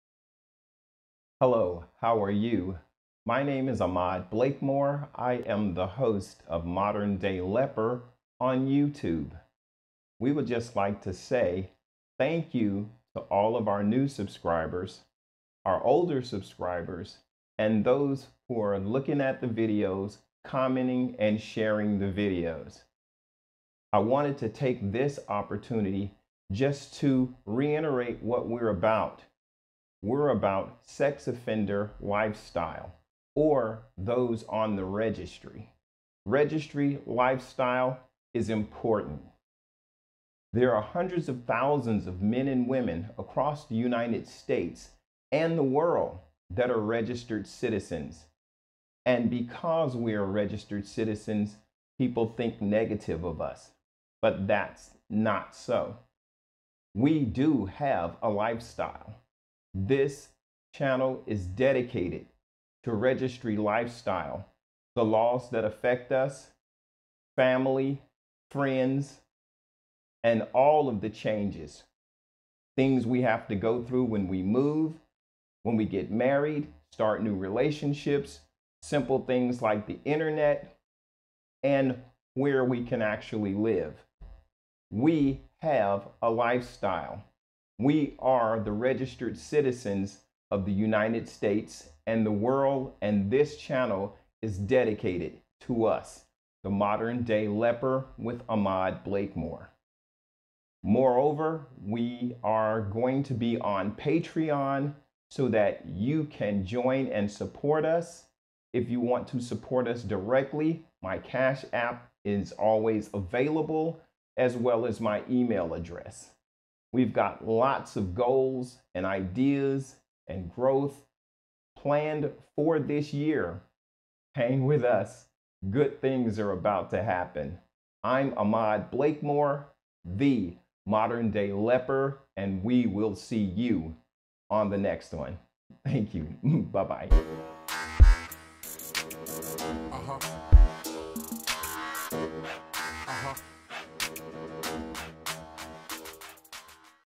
Sex Offenders Welcome
In this video, sex offenders welcome, Ahmad Blakemore, talks about why he started this channel and the plans he has for the future. Sex offenders welcome talks about sex offender lifestyles in a positive way. Sex offenders welcome abaord!